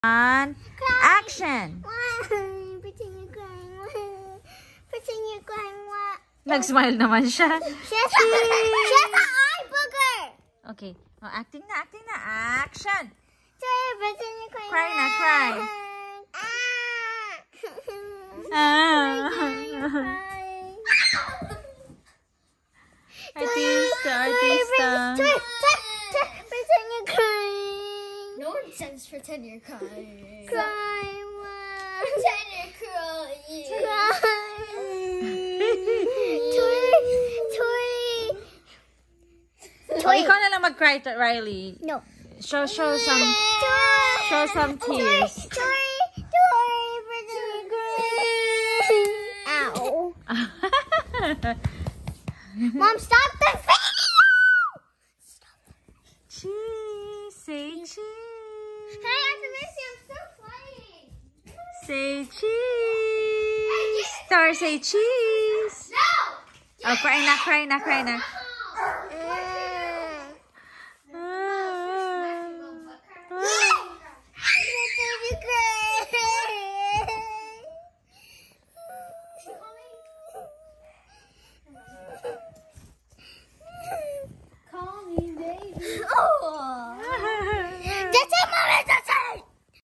Baby actresses
Tory and Sachi Rylee acting. Tory’s 21 months and Rylee’s 7 years old.